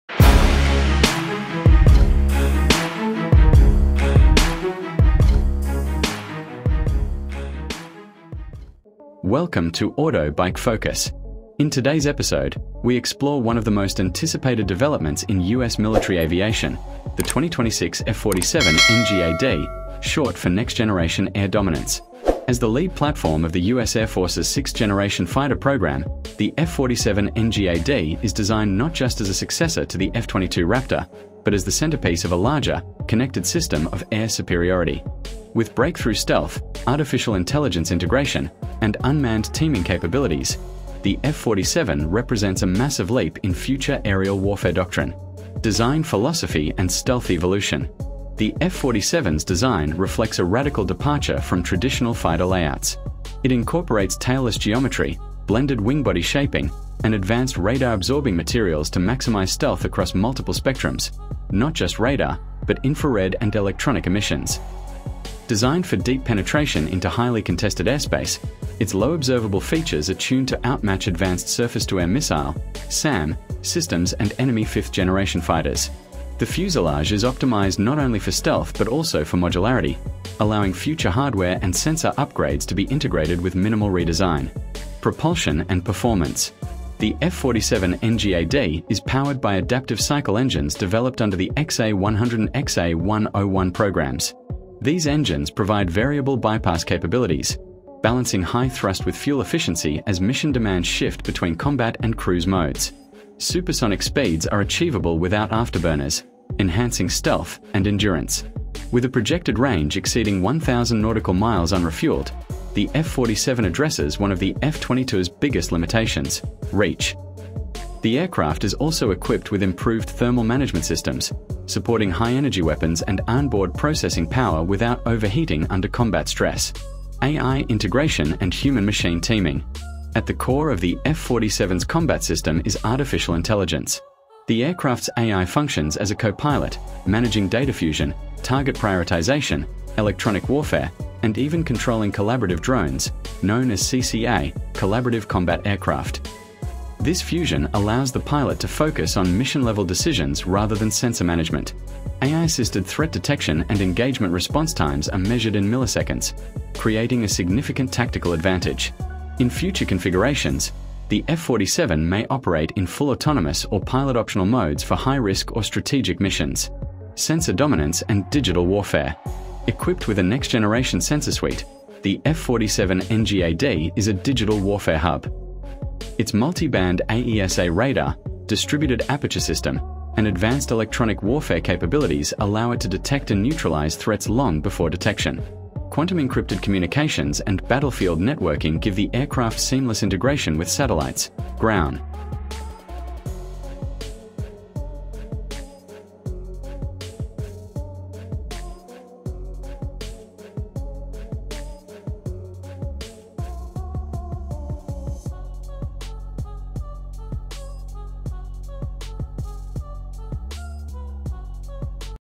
INSANE Reveal: 2026 F-47 NGAD Will Change Air Combat FOREVER
Description:
The future is here. The U.S. Air Force has officially unveiled the F-47 NGAD, the most advanced 6th-generation stealth fighter jet we've never seen before. With AI-assisted piloting, adaptive engines, and drone-wingmen capability, this jet is years ahead of anything flying today.

In this video, we break down the top 10 game-changing features of the F-47 NGAD, how it compares to the F-22 & F-35, and why rival nations are already worried.

👀 Stick around for #1 — it’s something NO ONE expected.

🔟 Top 10 Video Section Titles (Cover Titles):

#10: Cloaked in Plasma? The New Stealth Tech

#9: AI Pilot—Smarter Than a Human?!

#8: Hypersonic Speed Confirmed

#7: Drone Wingmen That Think for Themselves

#6: No Radar? No Problem

#5: Ghost Mode — Total Electronic Silence

#4: Laser Weapons: Fact or Fiction?

#3: Combat Range That Shocks Experts

#2: Rival Nations in Panic Mode

#1: The Secret That Wasn’t Supposed to Leak